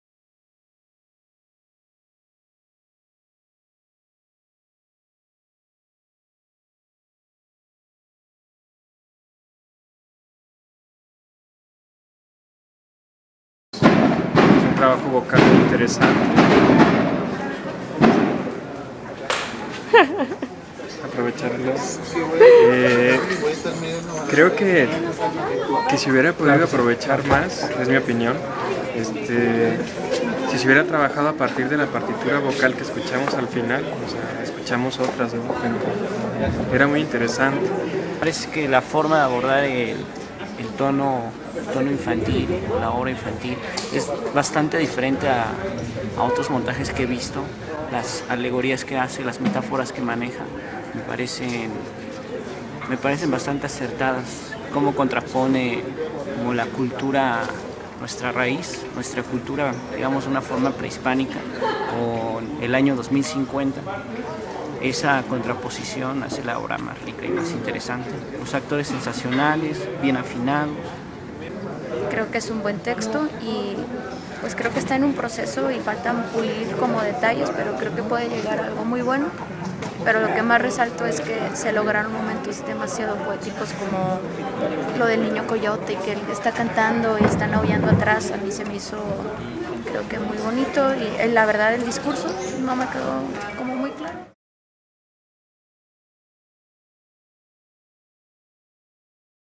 En la tumba de los dioses
Comentarios sobre la obra En la tumba de los dioses de Verónica Musalem, dirigida por Luis Martín Solís en el 10 Festival de la Joven Dramaturgia